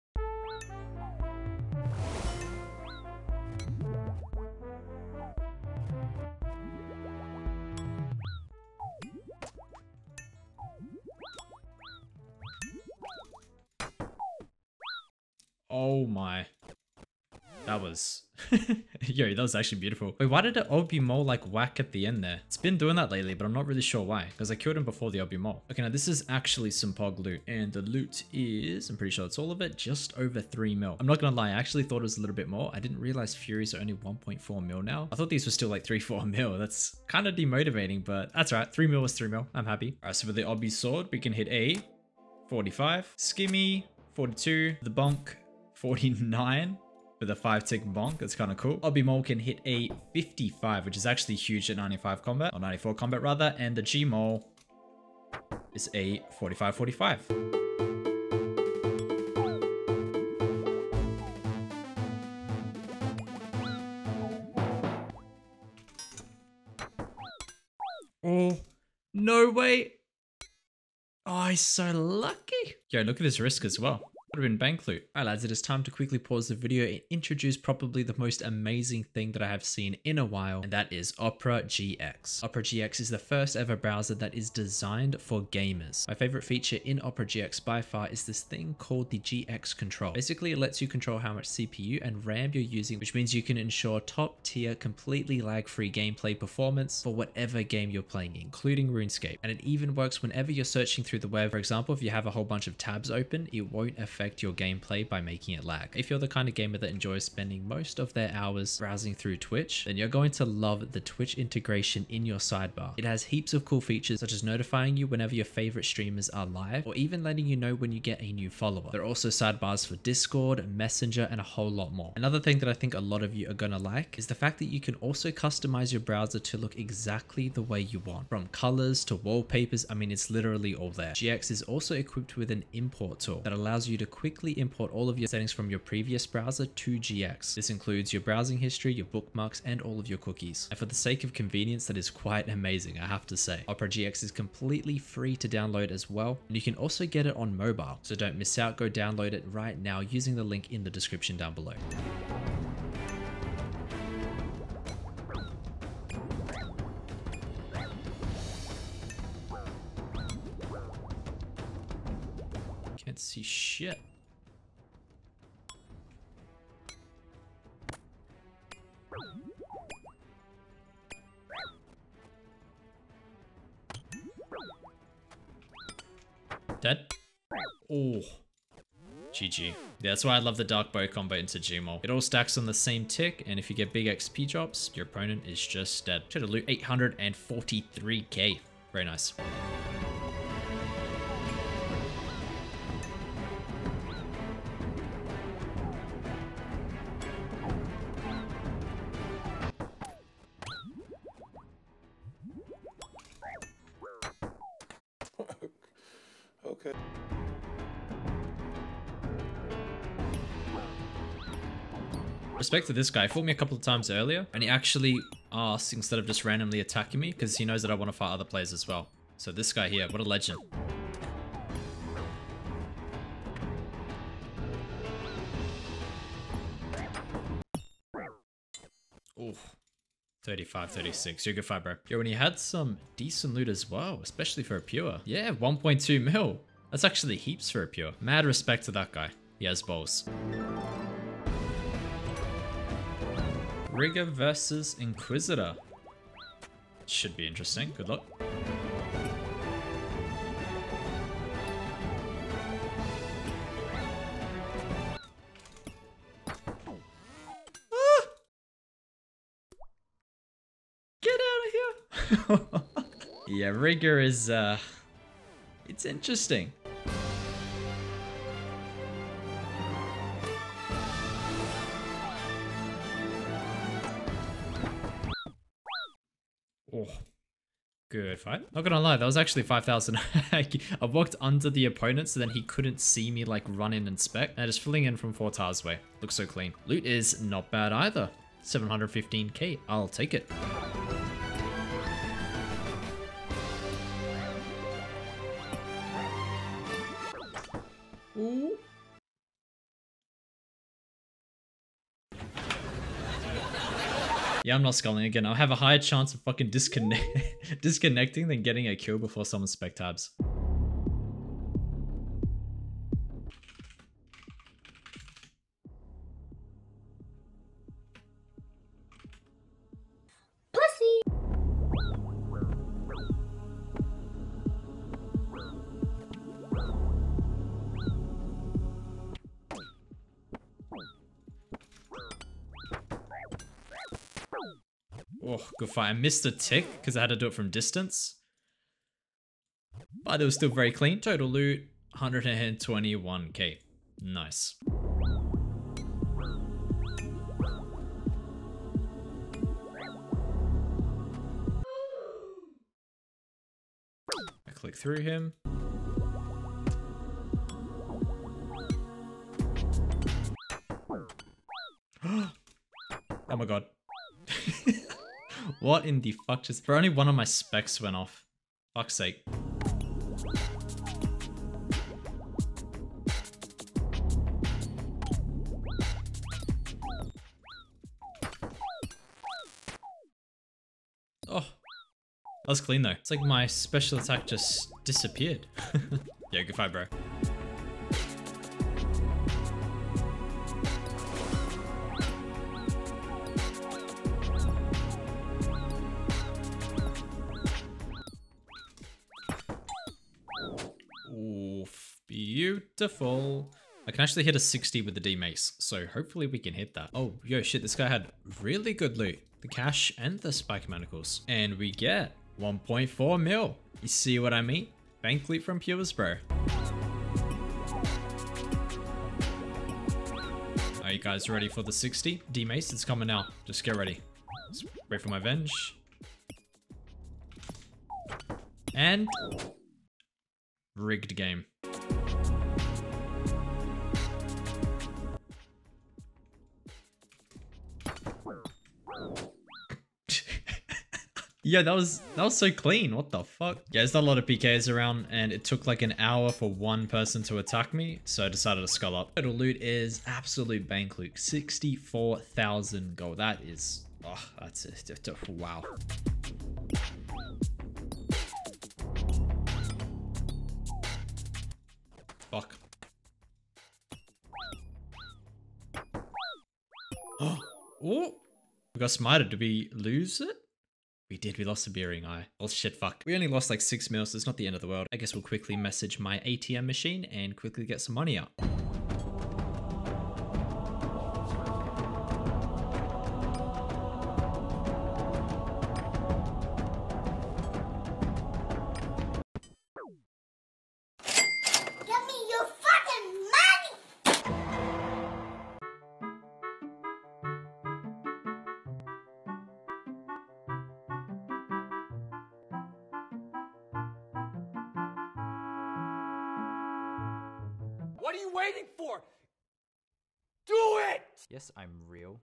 I Used Crush Weapons at Low Combat in PvP Worlds
0:00 - 1:15 Pking
1:15 - 2:31 OperaGX
2:31 - 7:53 Pking
7:53 - 8:32 Atm Machine LOL